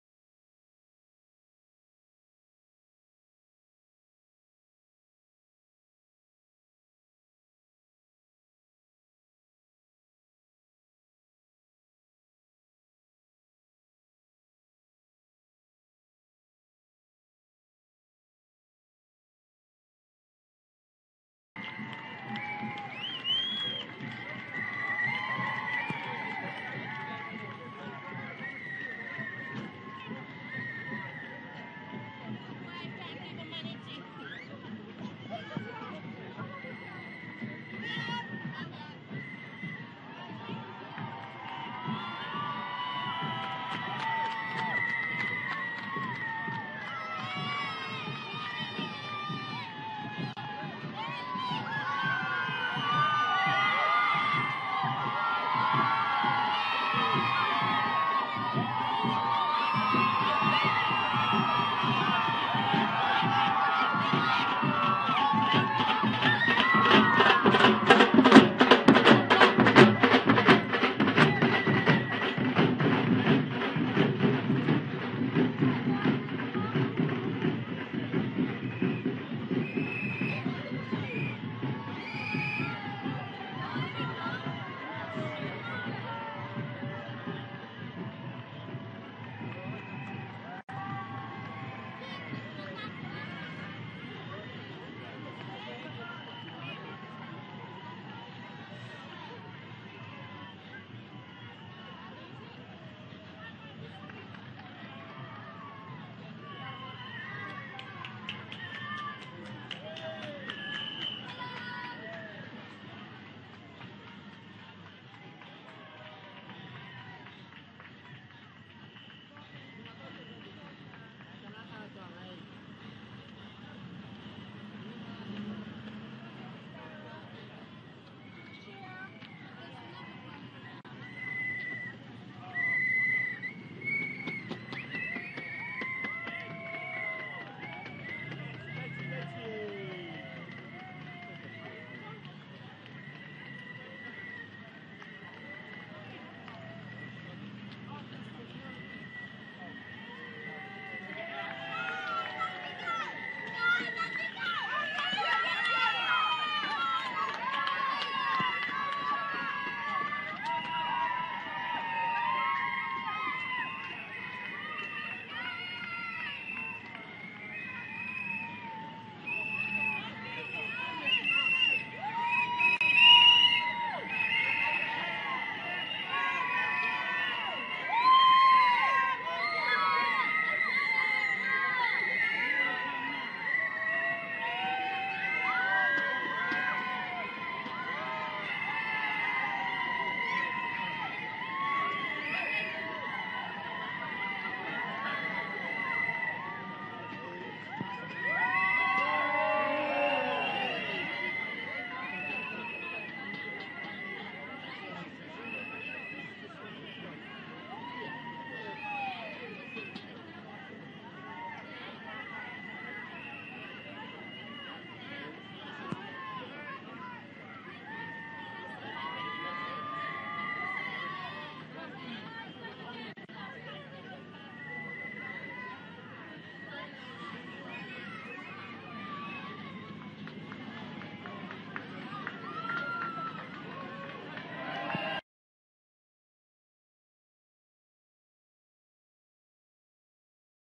matthew c
Matthew Church was part of the Olympic Torch Relay, Gravesend, 20th July 2012